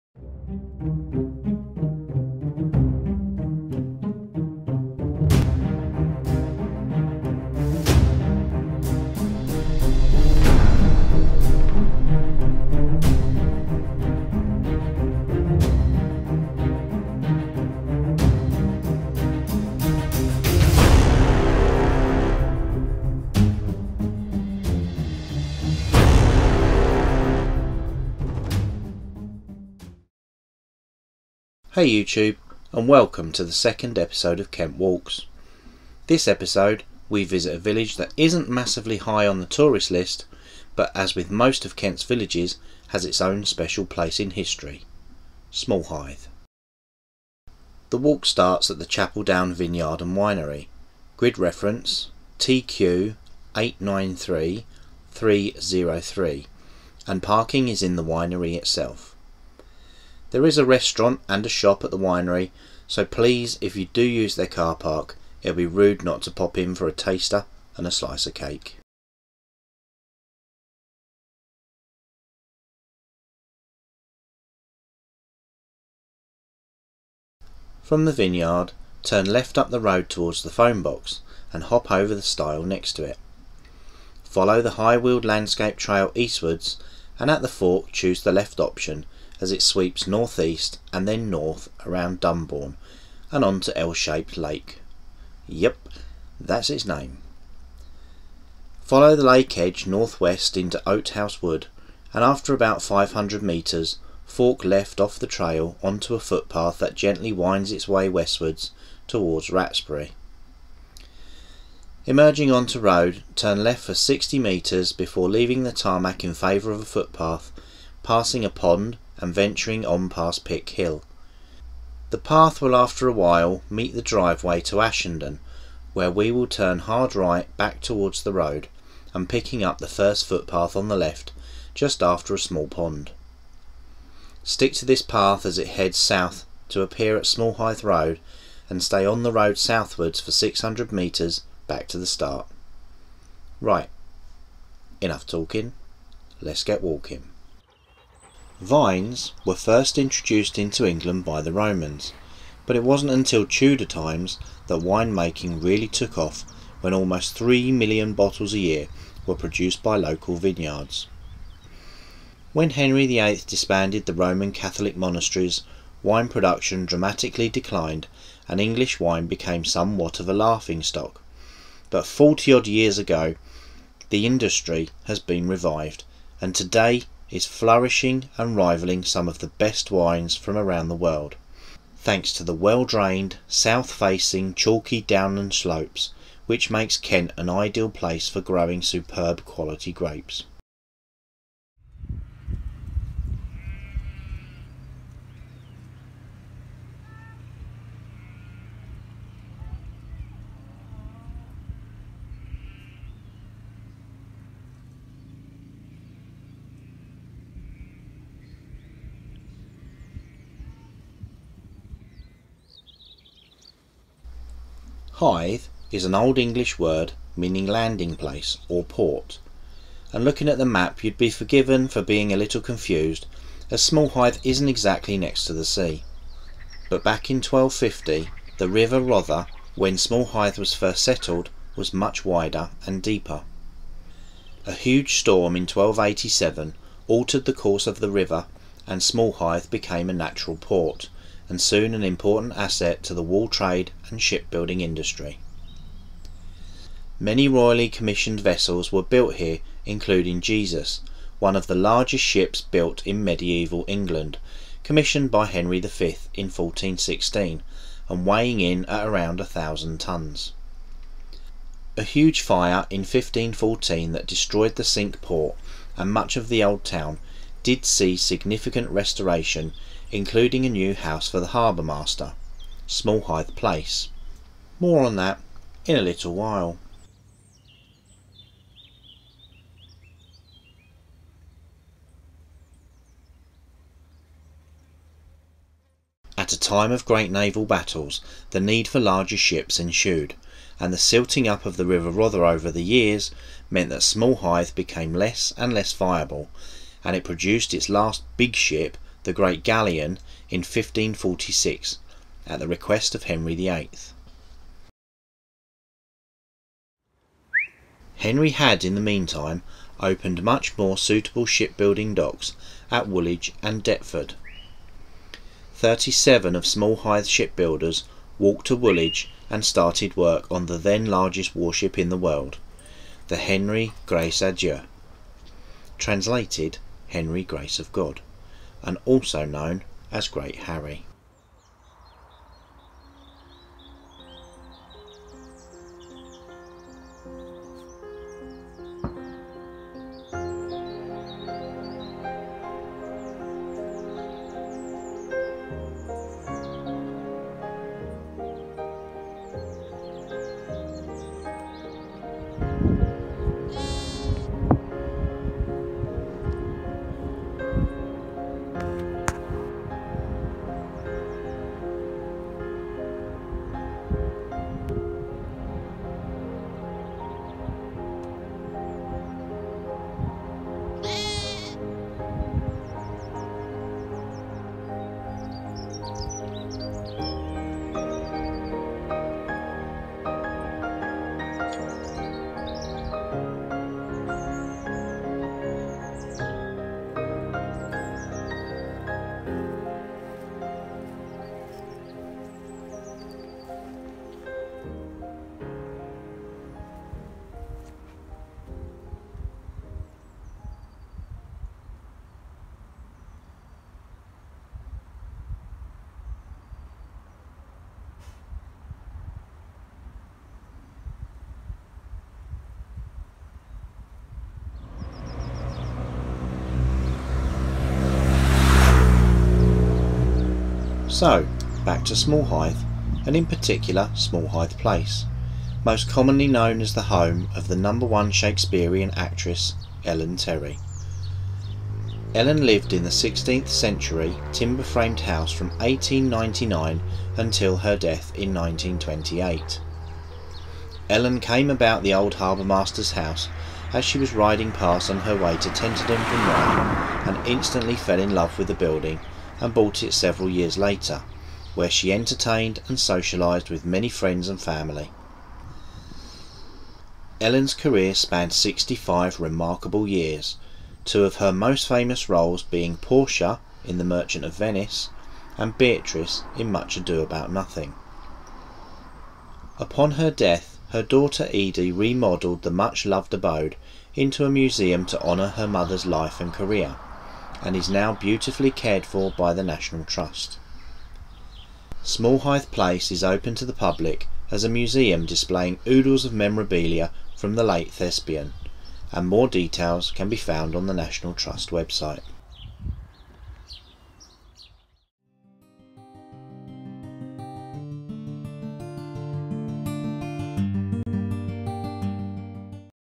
KENT Walks Episode 2 - Smallhythe
In this episode I visit the sleepy village of Smallhythe and discover it's remarkable history and venture through wooded valleys towards Tenterden.